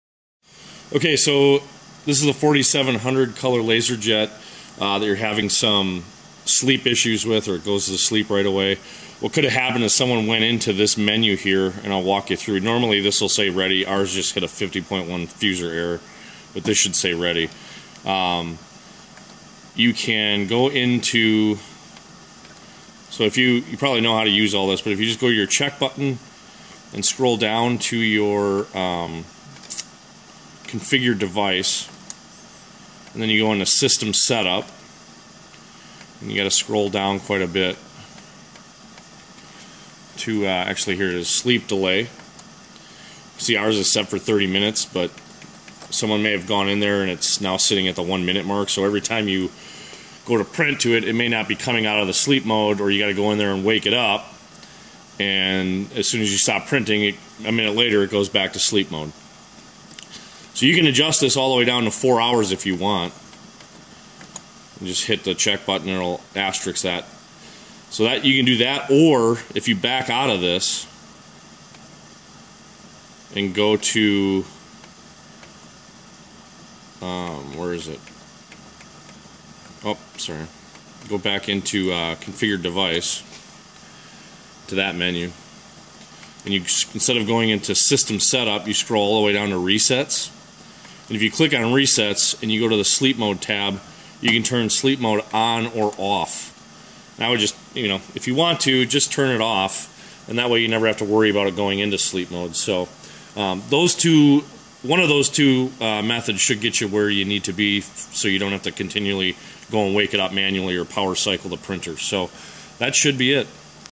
HP Color LaserJet 4700 Sleep Mode Manipulation
In this video we discuss how to adjust the sleep mode time on your HP Color LaserJet 4700 printer.  We also demonstrate how to remove or turn off sleep mode completely.